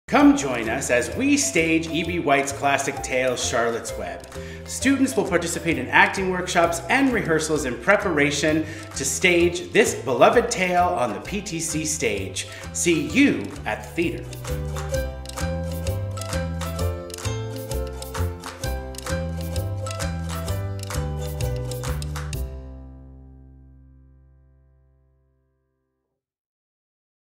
Charlotte’s Web
Classes begin January 25  Ages 8 to 18
Tuesday and Thursday from 4pm to 6:30pm

Come join us on stage this winter as we stage E.B. White’s classic, Charlotte’s Web! Students will take part in acting workshops and rehearsals in preparation to perform a play version of this beloved tale on the PTC stage. Tiny thespians will participate in twice weekly classes, and also participate in the show in small roles, as well as perform a special pre-show poem about life on the farm! For students who would like to participate behind the scenes, we will be offering a tech track where students can learn all about costumes, make-up, lights, sound, set design and more! We hope to see you soon!